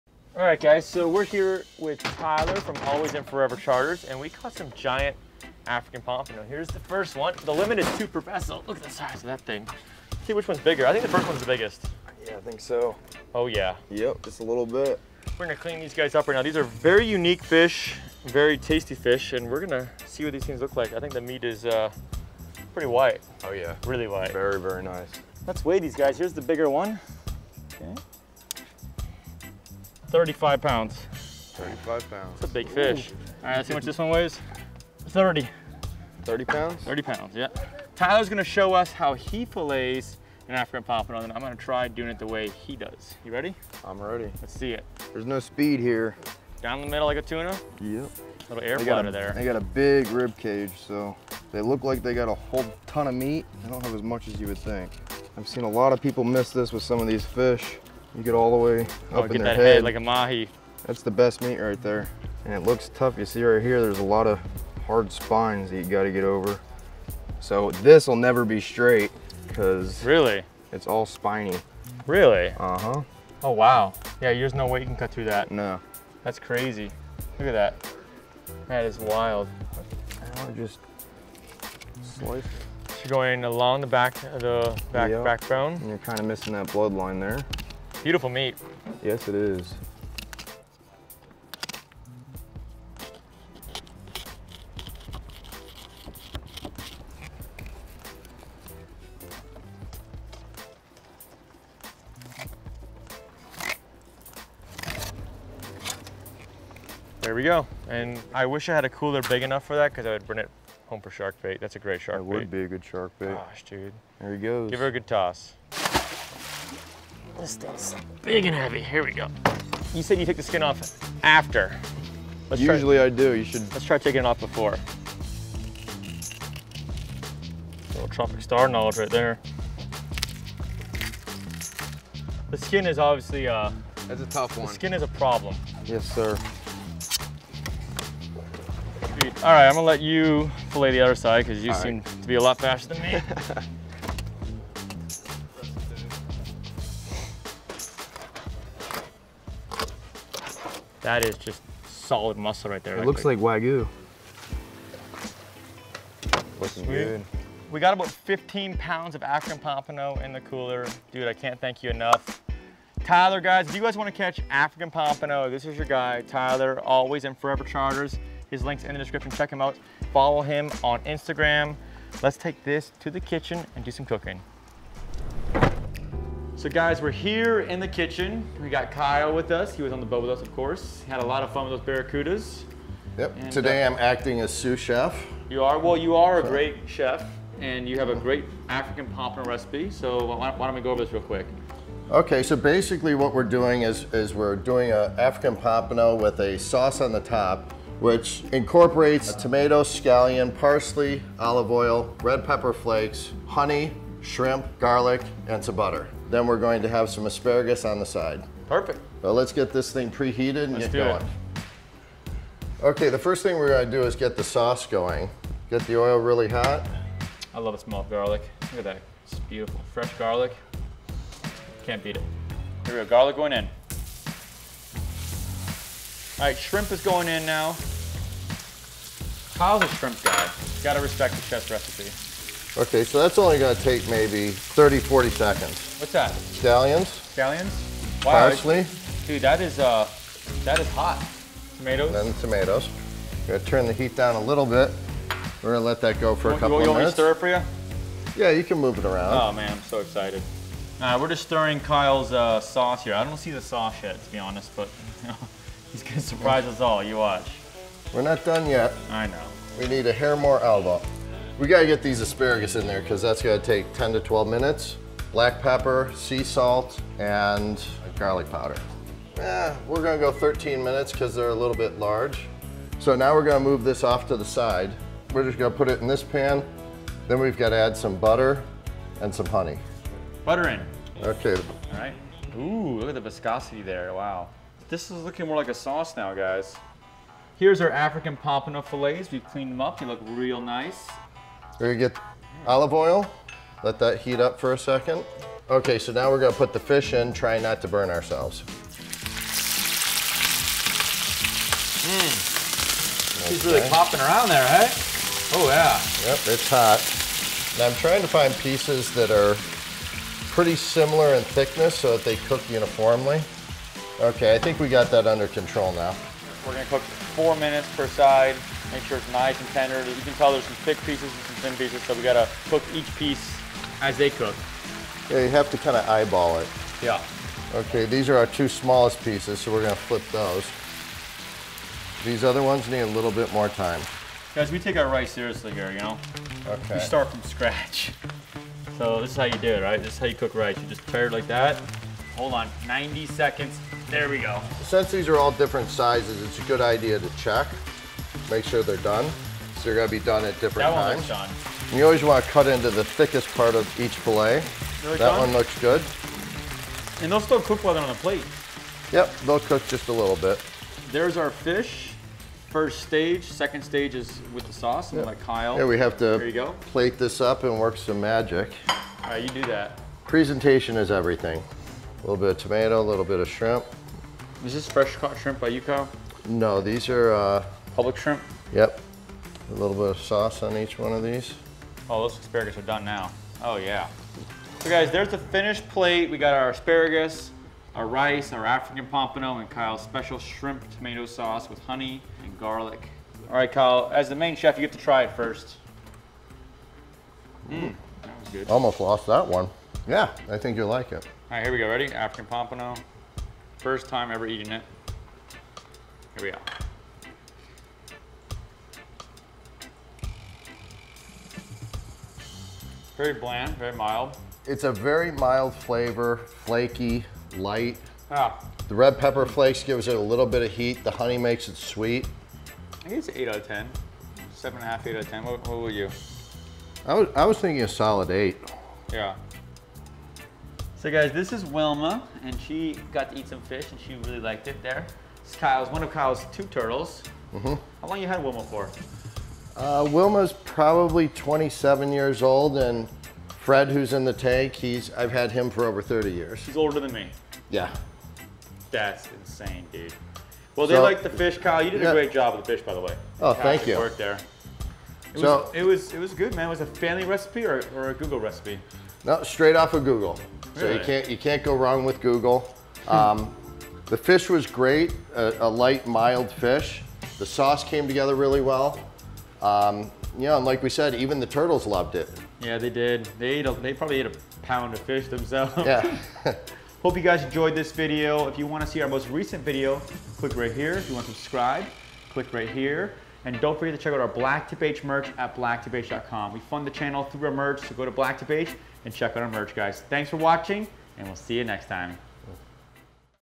GIANT African Pompano - Clean & Cook
In this episode of BlacktipH, we show you how to clean and cook a giant African Pompano! African Pompano are very difficult to clean due to their unique bone structure. Capt. Tyler DeGraff shows us the proper techniques on how to clean an African Pompano. Kyle Woodhead is our head chef, and he hooks the African Pompano with a Honey Shrimp sauce.

• Follow Tyler DeGraff on Instagram → @capt_tdegraff

▬▬▬▬▬▬▬ ABOUT BLACKTIPH ▬▬▬▬▬▬▬

▬▬▬▬▬▬▬ BLACKTIPH PRODUCTS ▬▬▬▬▬▬▬

▬▬▬▬▬▬▬ MUSIC SOURCES ▬▬▬▬▬▬▬

• Pond5
• Audio Jungle
• PremiumBeat

▬▬▬▬▬▬▬ PREVIOUS EPISODE ▬▬▬▬▬▬▬